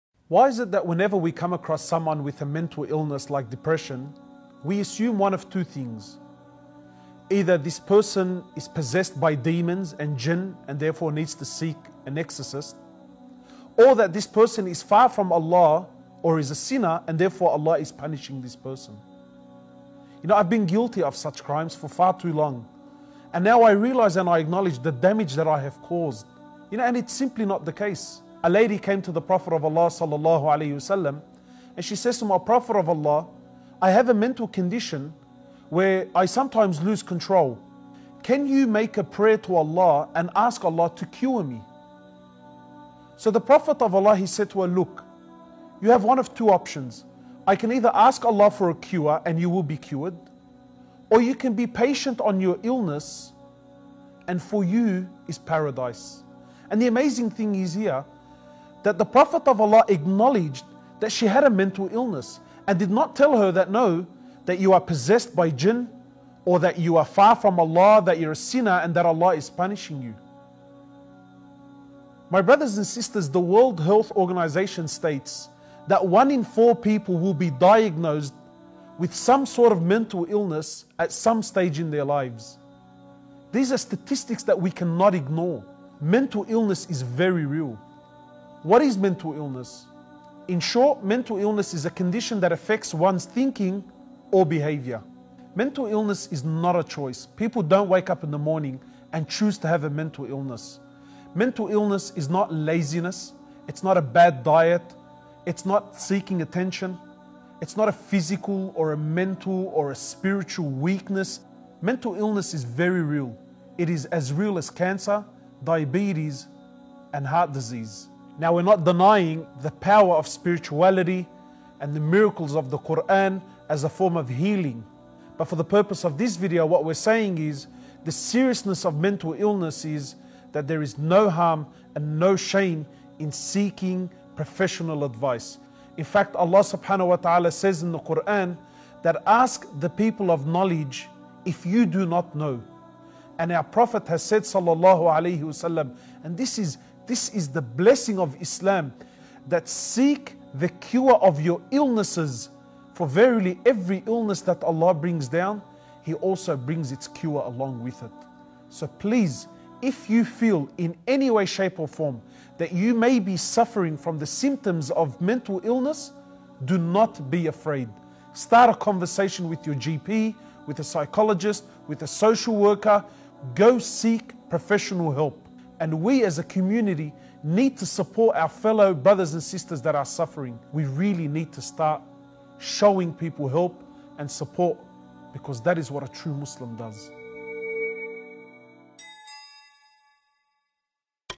Gangguan Mental Menurut Pandangan Islam
Gangguan Mental sering kali di kesampingkan lho, bahkan gangguan mental disebut-sebut sebagai kutukan.

Daripada kita salah sangka tentang gangguan mental, mending nonton dulu videonya yuk!